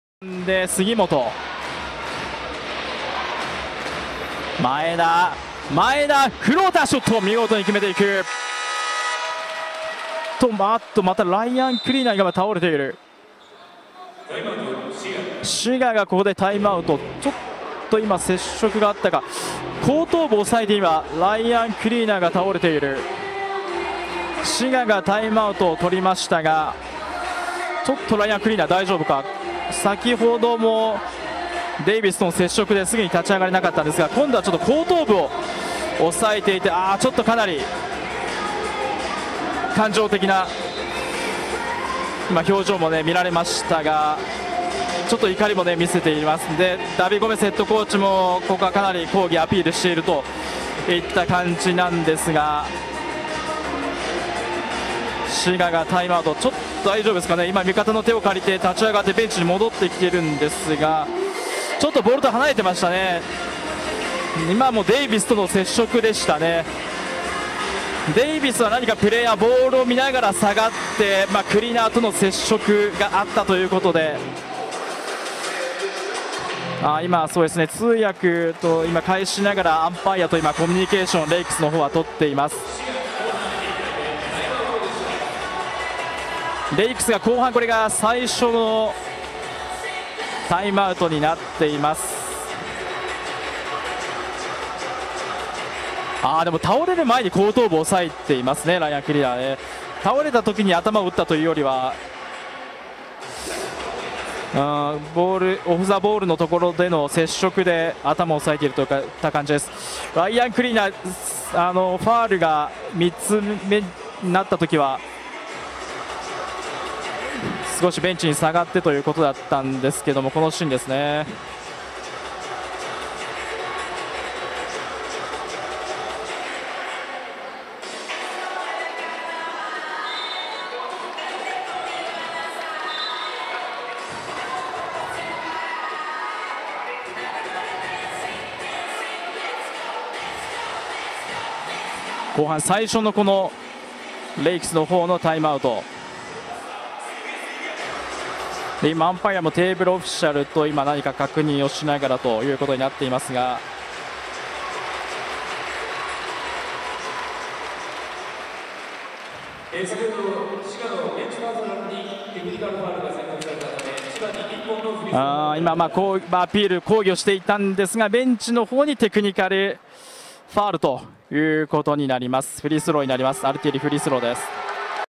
【誤審】2023年10月29日 滋賀レイクスvsアルティーリ千葉 アレックス・デイビスのヘッドラリアット【Bリーグ・B2】
ゴール下の滋賀4番ライアン・クリーナーの背後からA千葉9番アレックス・デイビスが頭にラリアットをかましてクリーナーが転倒
審判全員気付いておらず、滋賀レイクスのダビー・ゴメスヘッドコーチがタイムアウトをとり猛抗議
猛抗議に対して滋賀レイクスのベンチに審判団がテクニカルファウル

クルーチーフ 早﨑康祐
ファーストアンパイア 岩月遼司
セカンドアンパイア 砂川卓嗣